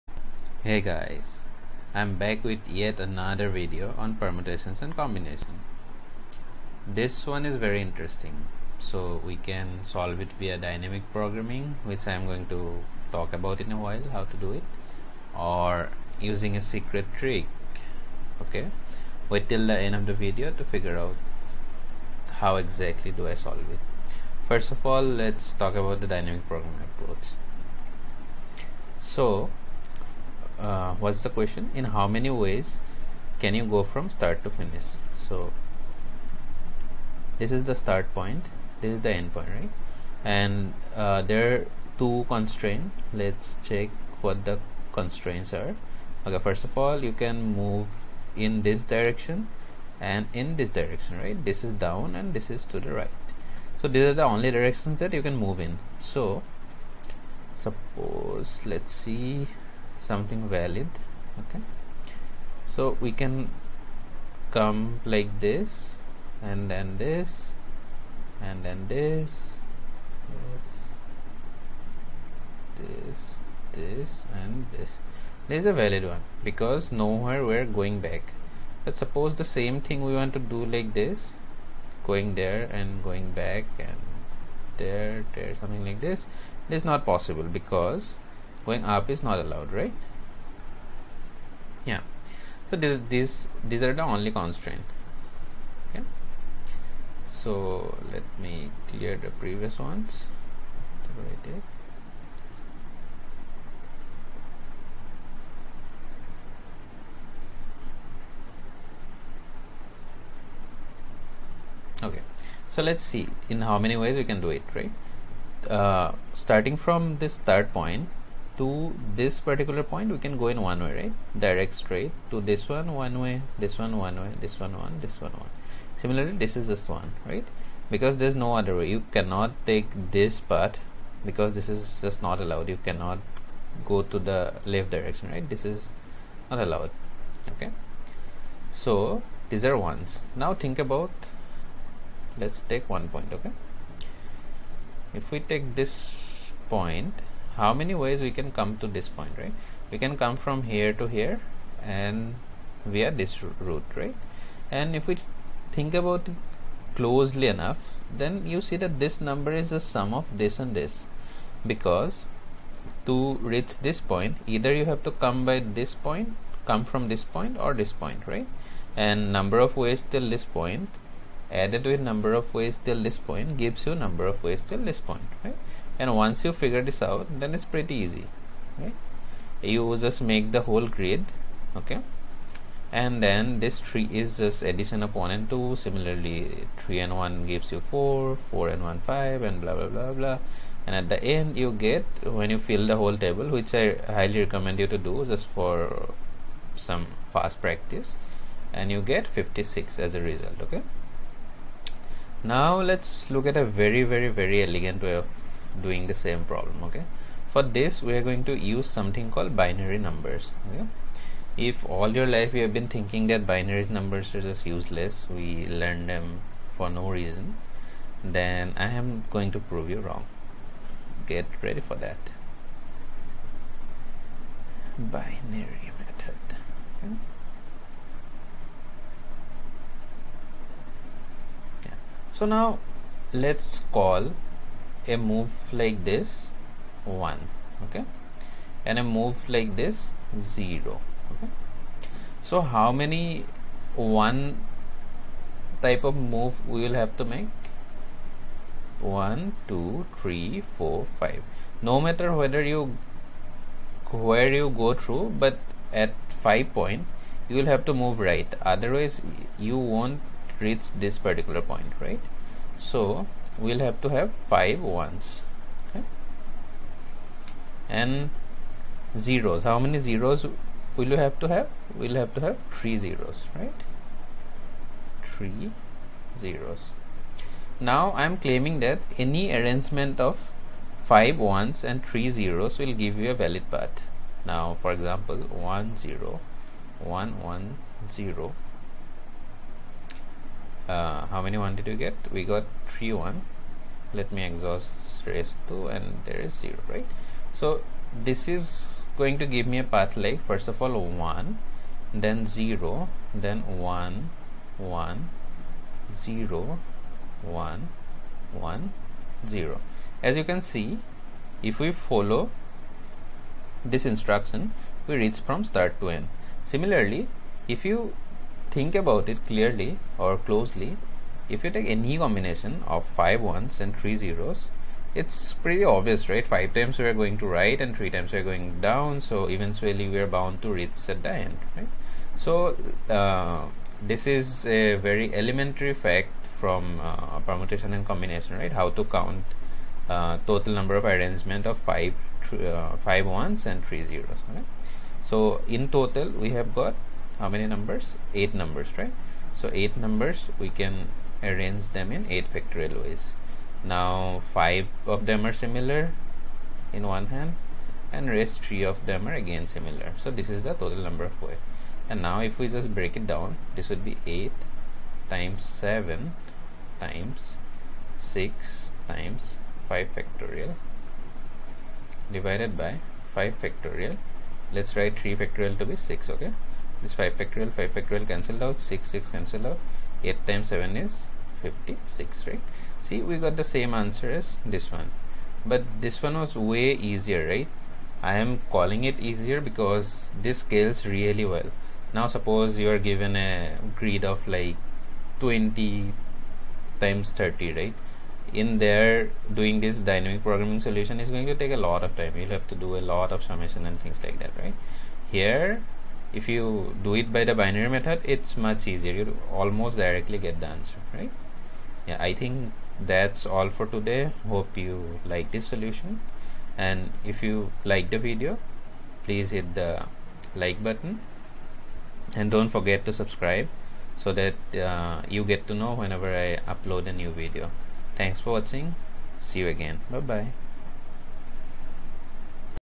Dynamic Programming | Counting path | Maze |short and elegant trick | Permuation & Combination
In this problem we explore the number of ways you can navigate through a maze given a particular set of constraints. We also explore a really beautiful solution in addition to the dynamic programming approach. Highly relevant Permutations and Combinations problem for JEE Advanced.